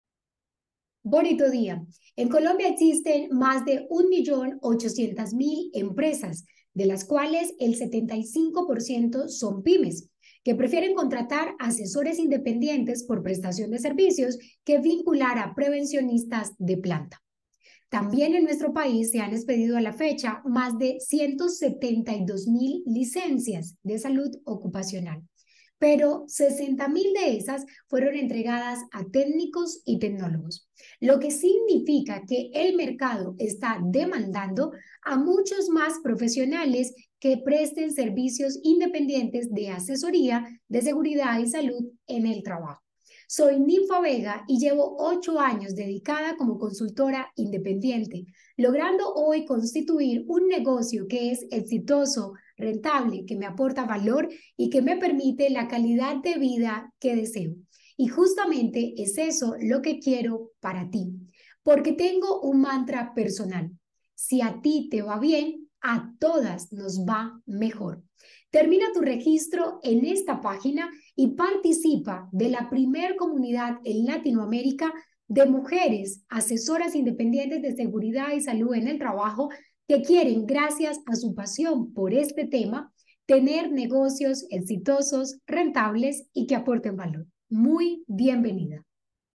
Aprende a facturar mínimo 4 millones al mes con tus asesorías de seguridad y salud
💰🚀 Descubre cómo generar ingresos mínimos de 4 millones al mes con tus servicios de seguridad y salud.
📅 Participa en nuestro entrenamiento de NEGOCIOSST del 16 de enero.

🌟 Dedica 4 noches a mejorar tus resultados y a establecer la estructura adecuada para hacer del 2024 tu año más exitoso.